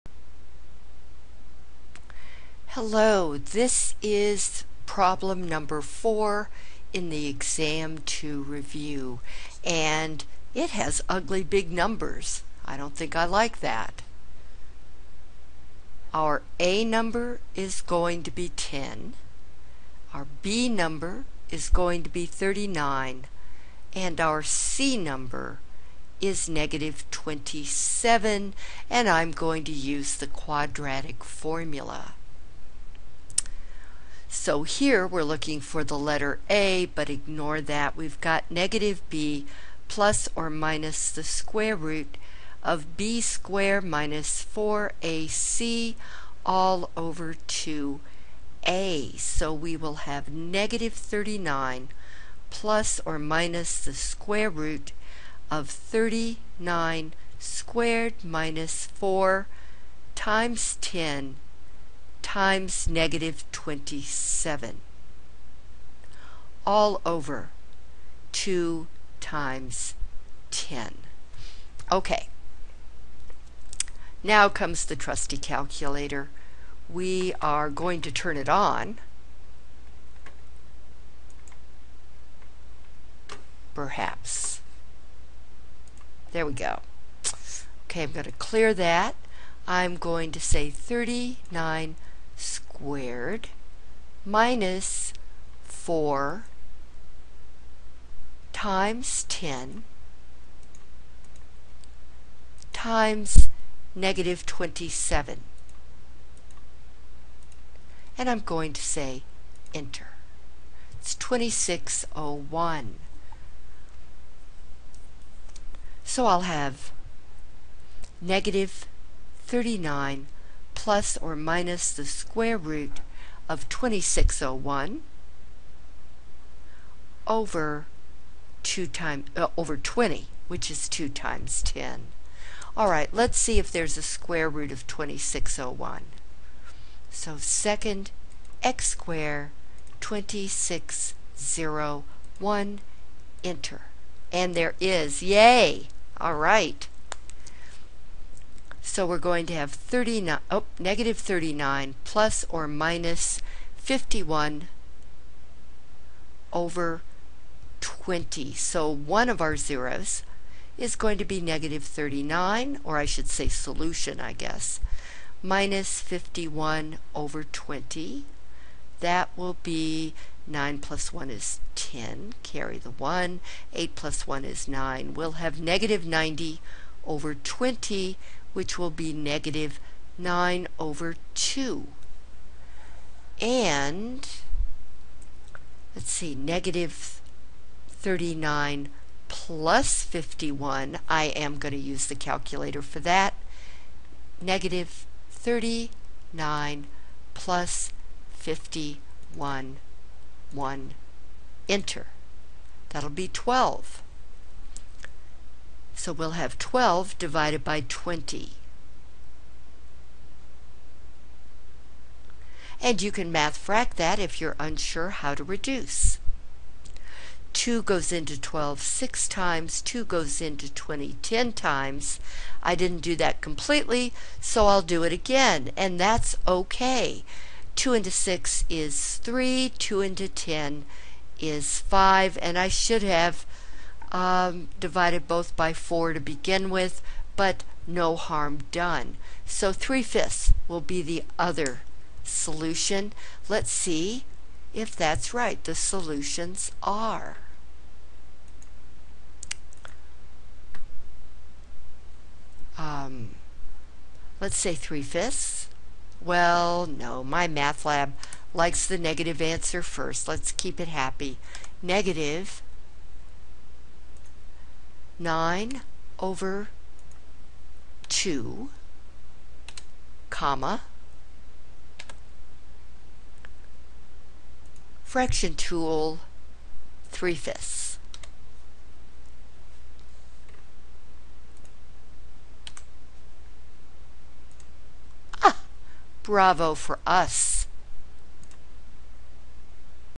Use Quadratic Formula
In this video, we use the quadratic formula instead of factoring to solve a quadratic equation.